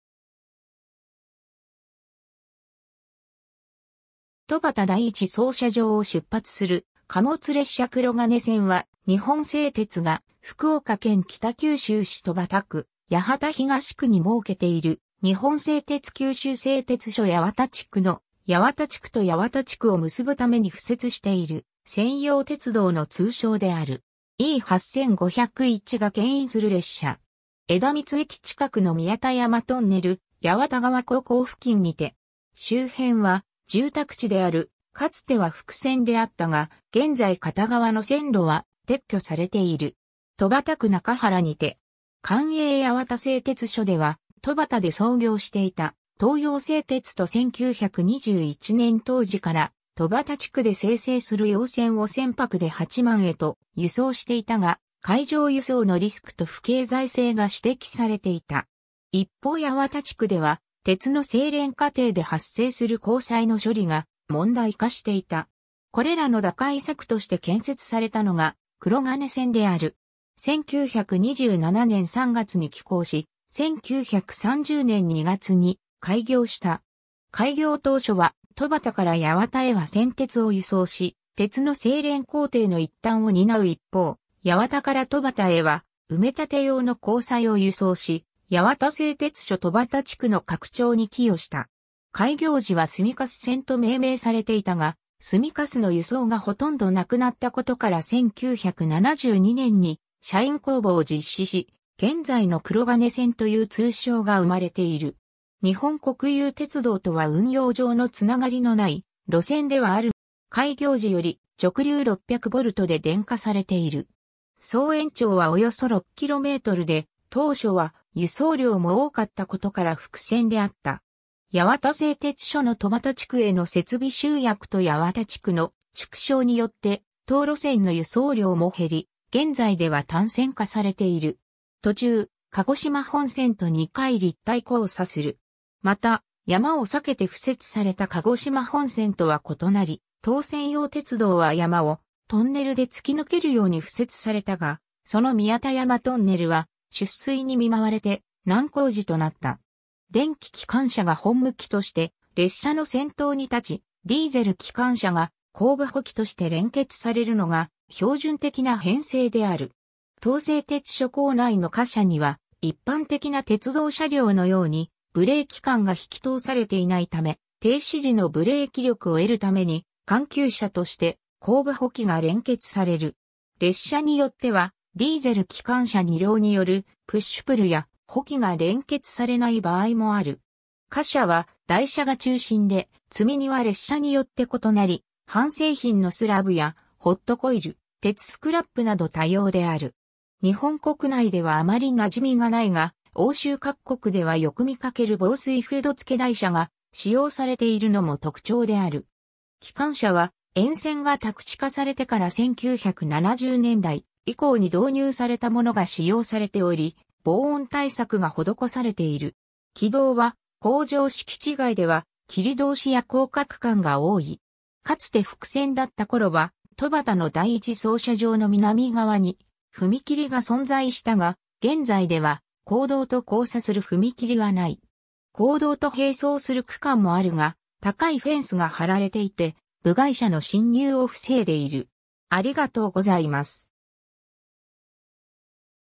くろがね線
戸畑第一操車場を出発する貨物列車（2009年3月） くろがね線（くろがねせん）は、日本製鉄が福岡県北九州市戸畑区・八幡東区に設けている日本製鉄九州製鉄所八幡地区の、八幡地区（戸畑）と八幡地区（八幡）を結ぶために敷設している専用鉄道の通称である。
 E8501が牽引する列車。
枝光駅近くの宮田山トンネル八幡側坑口付近にて。
周辺は住宅地である（2009年2月） かつては複線であったが、現在片側の線路は撤去されている。
戸畑区中原にて。
（2010年11月） 官営八幡製鐵所では、戸畑で操業していた東洋製鐵と1921年当時から、戸畑地区で生成する熔銑を船舶で八幡へと輸送していたが、海上輸送のリスクと不経済性が指摘されていた。
一方八幡地区では、鉄の製錬過程で発生する鉱滓の処理が問題化していた。
これらの打開策として建設されたのが、くろがね線である。
 1927年3月に起工し、1930年2月に開業した。
開業当初は、戸畑から八幡へは銑鉄を輸送し、鉄の製錬工程の一端を担う一方、八幡から戸畑へは埋め立て用の鉱滓を輸送し、八幡製鉄所戸畑地区の拡張に寄与した。
 開業時は炭滓線（たんさいせん）と命名されていたが、炭滓の輸送がほとんどなくなったことから1972年に社員公募を実施し、現在のくろがね線という通称が生まれている。
 日本国有鉄道（現：九州旅客鉄道、日本貨物鉄道）とは運用上の繋がりのない路線ではあるものの、国鉄線を介して原料・製品輸送を行う都合上、軌間・車両限界については国鉄在来線と同等である。
開業時より直流600Vで電化されている。
総延長はおよそ6kmで、当初は輸送量も多かったことから複線であった。
八幡製鉄所の戸畑地区への設備集約と八幡地区の縮小によって当路線の輸送量も減り、現在では単線化されている。
途中、鹿児島本線と二回立体交差する（スペースワールド駅～枝光駅間、九州工大前駅の東方）。
また山を避けて敷設された鹿児島本線とは異なり、当専用鉄道は山をトンネルで突き抜けるように敷設されたが、その宮田山トンネル（総延長1,179m）は出水に見舞われて難工事となった。
 電気機関車が本務機として列車の先頭にたち、ディーゼル機関車が後部補機として連結されるのが標準的な編成である。
当製鉄所構内の貨車には、一般的な鉄道車両のようにブレーキ管が引き通されていないため、停止時のブレーキ力を得るために緩急車として後部補機が連結される。
列車によっては、ディーゼル機関車2両によるプッシュプルや、補機が連結されない場合もある。
 貨車は台車（国鉄でいう長物車）が中心で、積荷は列車によって異なり、半製品のスラブやホットコイル、鉄スクラップ（鋼矢板や鉄道用レール）など多様である。
日本国内ではあまり馴染みが無いが欧州各国ではよくみかける防水フード付台車が使用されているのも特徴である。
 機関車は、沿線が宅地化されてから1970年代以降に導入されたものが使用されており、防音対策が施されている。
軌道は工場敷地外では切通や高架区間が多い。
かつて複線だった頃は戸畑の第一操車場の南側に踏切が存在したが、現在では公道と交差する踏切はない。
公道と並走する区間もあるが、高いフェンスが張られていて、部外者の侵入を防いでいる。